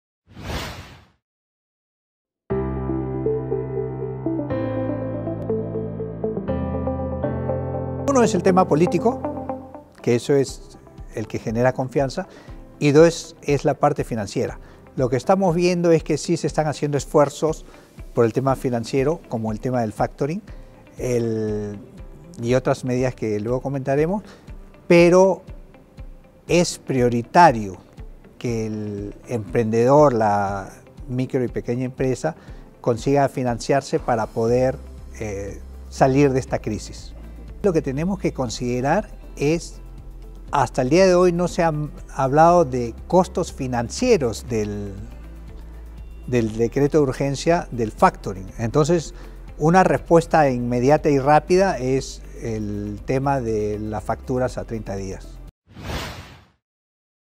Entrevista a Percy Krapp - ¿Qué medidas podrían ayudar a terminar de reactivar a las pymes?
Entrevista a Percy Krapp, presidente del Gremio de la Pequeña Empresa de la Cámara de Comercio de Lima.